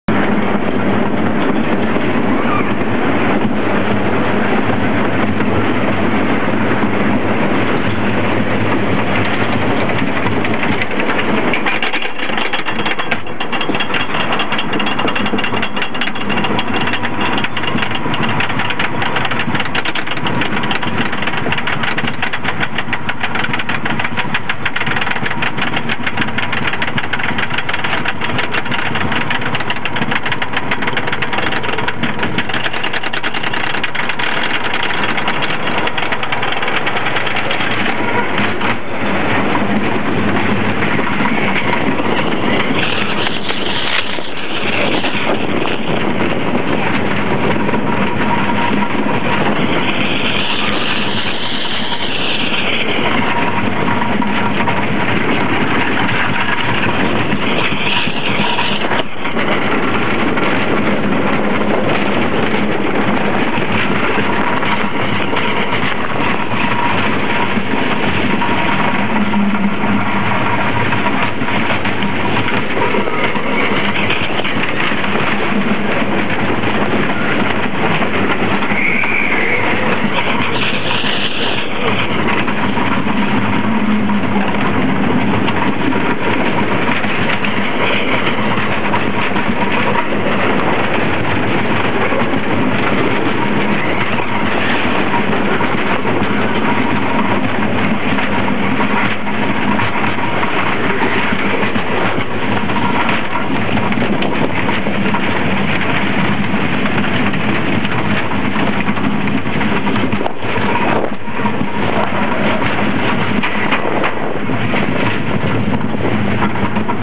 White Canyon ホワイト キャニオン (よみうりランド) POV 4 (front row)
Wooden roller coaster White Canyon on ride 4
Builder : TOGO (東洋娯楽機), Roller Coaster Corporation of America
Closed at 14th Jan 2013
Yomiuri Land, Inagi Tokyo/Kawasaki Kanagawa, Japan
ジェットコースター 絶叫マシン Roller Coaster